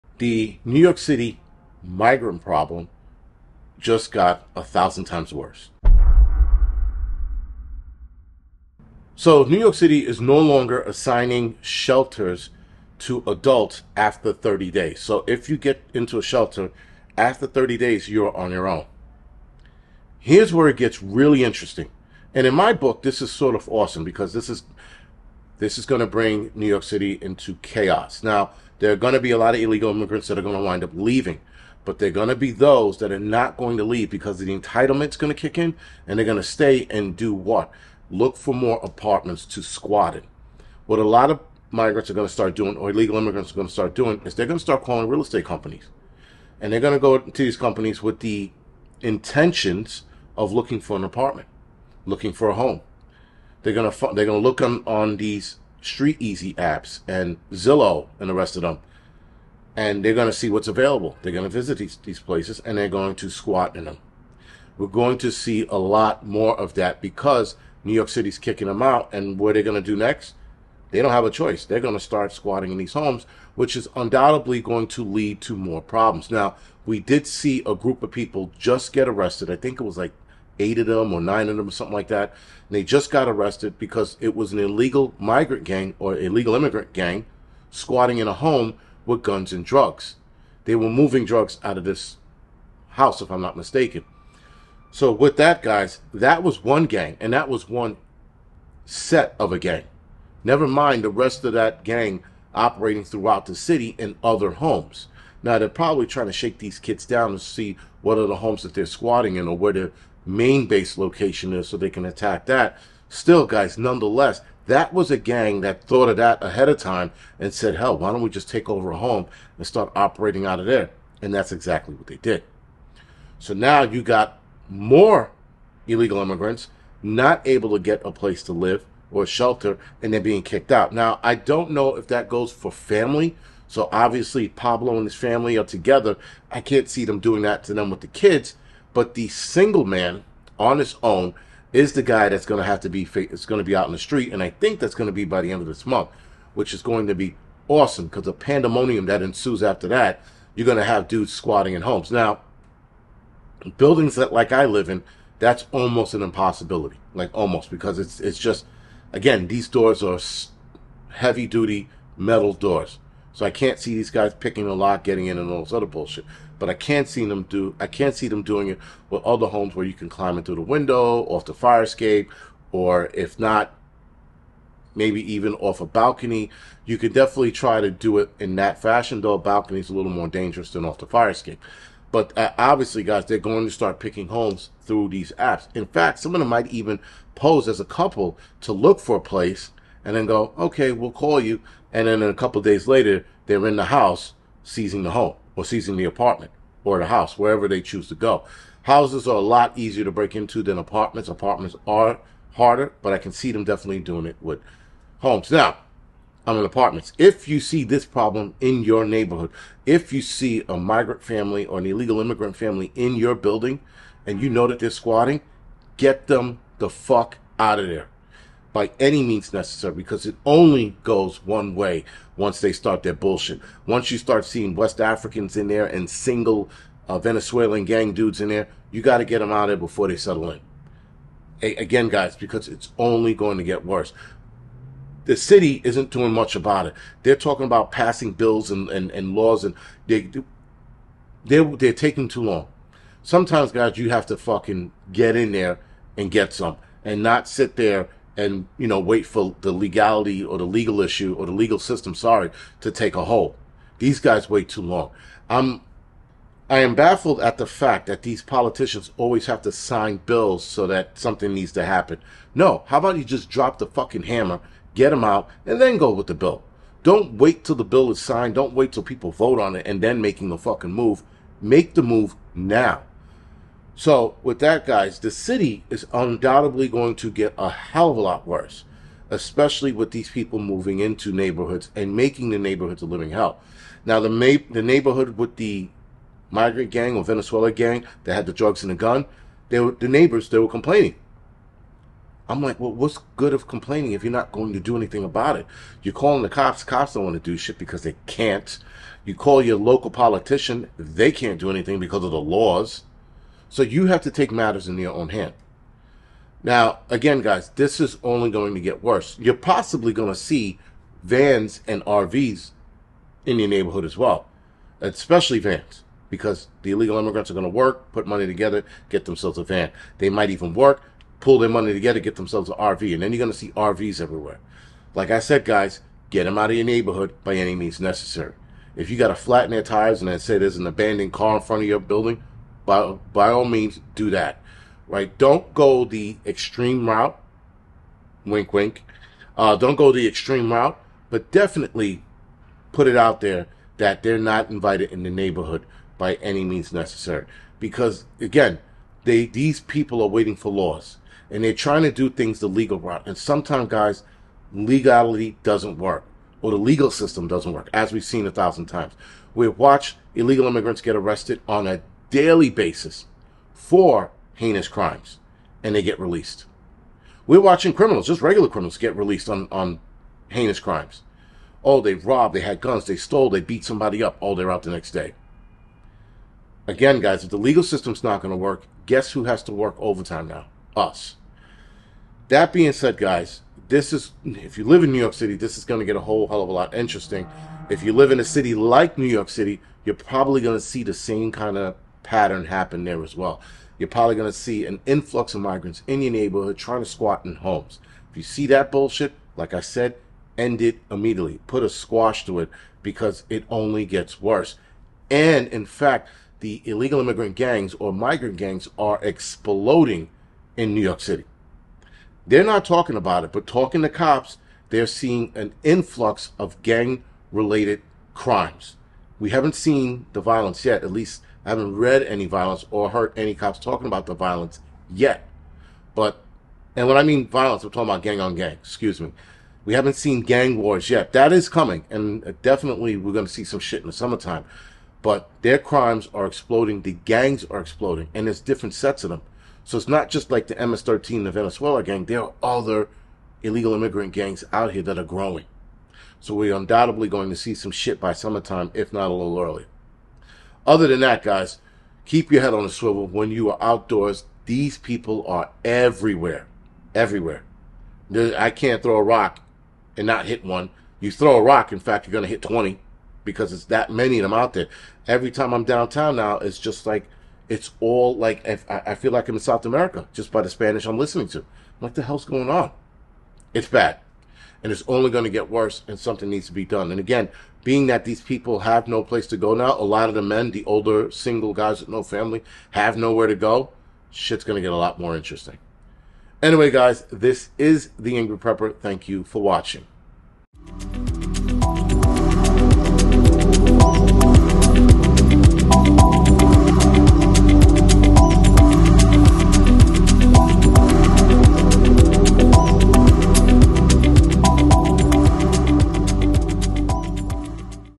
NYC Migrant Problem Just Got Worse…
On The Angry Prepper, we are going to talk about how NYC Migrant Problem Just Got Worse.  This is going to get very weird, very fast.  This isn't looking good for any people who came here looking for hand outs.

Amazon Store Front: